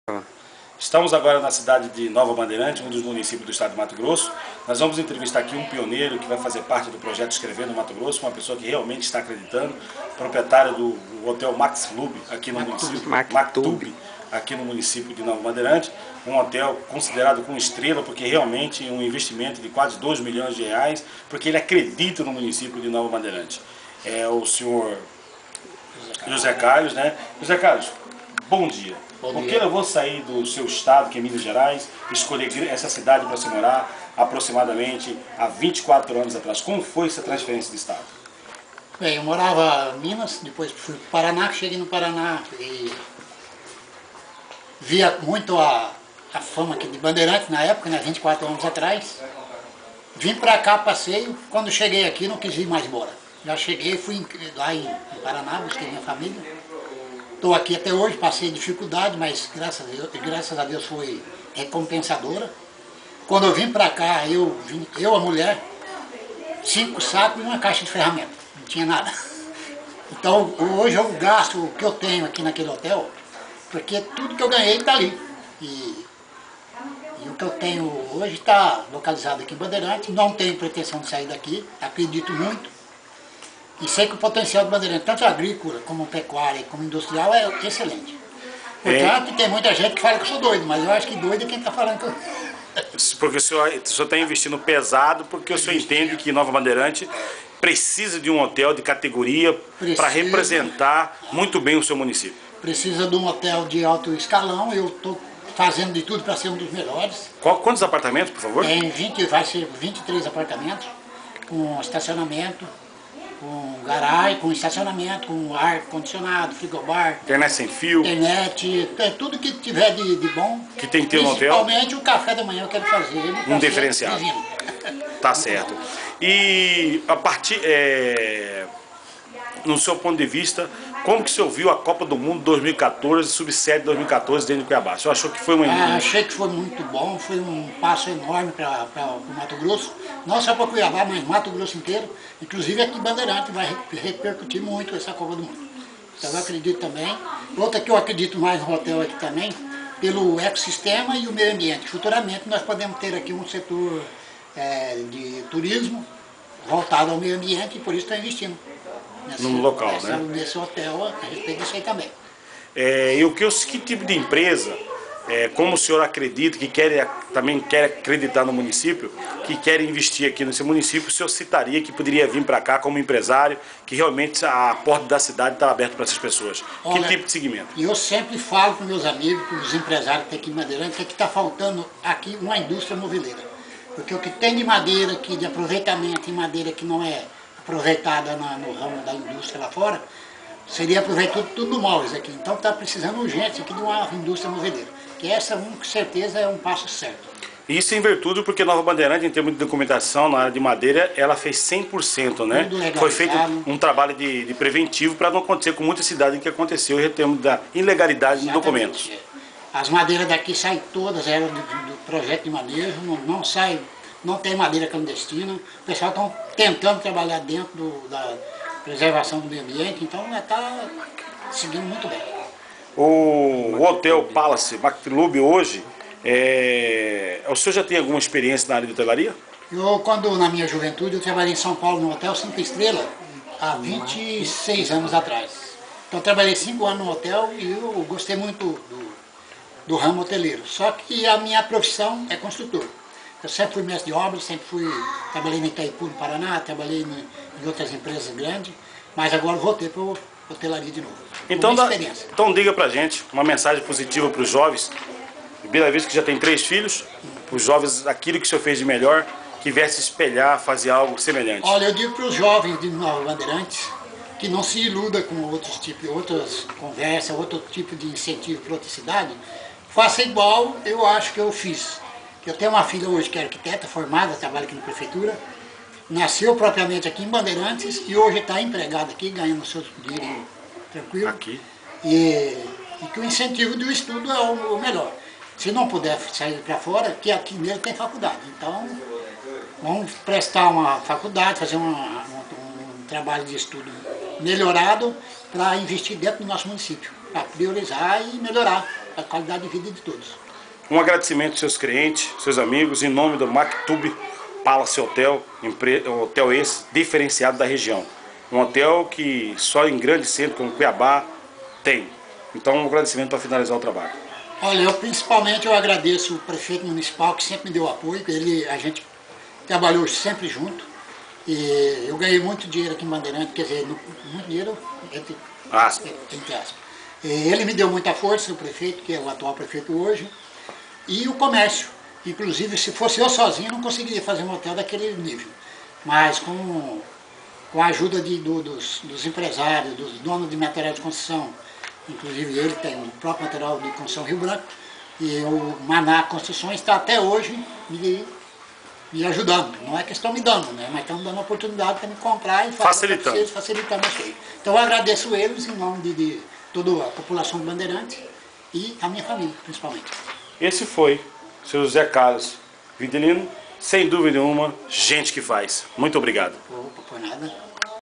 Maktube Hotel Empresário José Carlos Vidilino
Entrevista com José Carlos Vidilino, Empresário da Makitube Hotel
em Nova Bandeirantes-Mt.